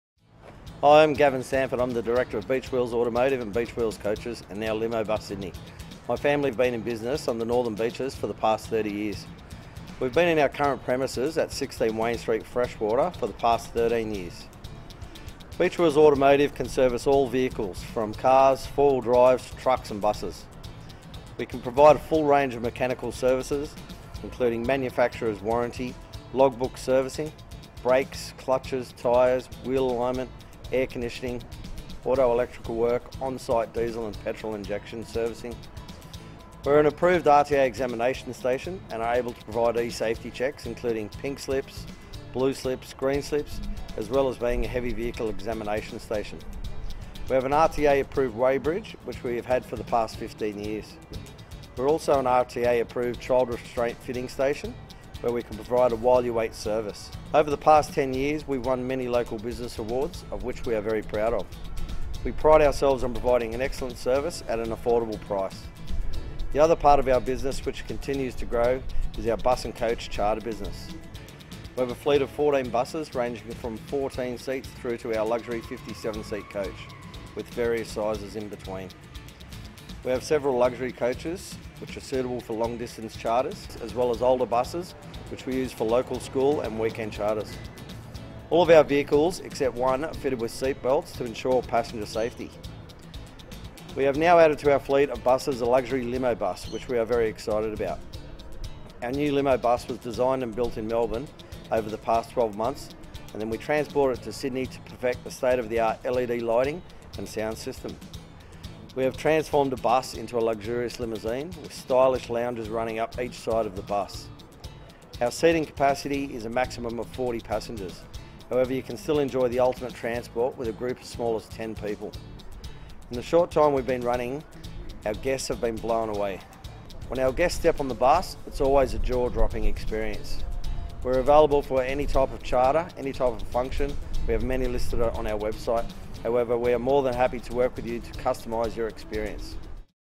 BeachWheels Automotive and Coaches
Beachwheels Automotive is located at Freshwater on Sydney's beautiful Northern Beaches. We have more than 25 years knowledge, which provides a vast range of Automotive Services from the family car or 4wd to trucks and buses. Beachwheels
have won numerous Customer Service Awards which is testament to the dedication of their outstanding staff who continually strive for excellence when attending to customer's vehicles. So for all your auto and mechanical requirements we look forward to seeing you at Beachwheels.